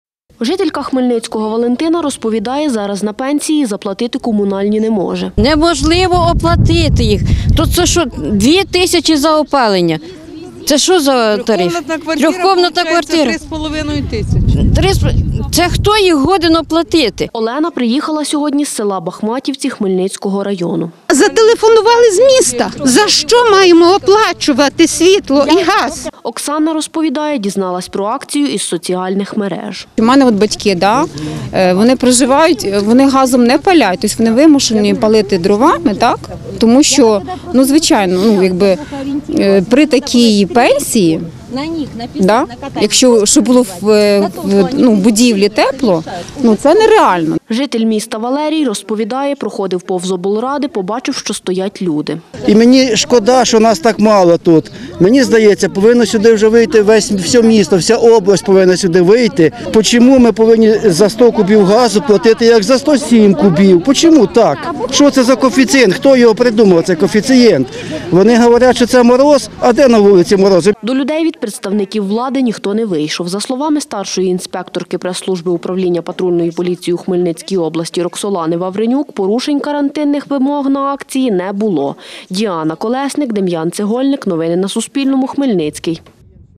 У Хмельницькому протестували проти підвищення тарифів на газ та електроенергію
Акція протесту проти підвищення комунальних платежів відбулась сьогодні у Хмельницькому біля Будинку рад. Як розповіла Суспільному старша інспекторка пресслужби управління патрульної поліції у Хмельницькій області Роксолана Вавренюк, під стінами обласної ради зібралось близько сімдесяти людей.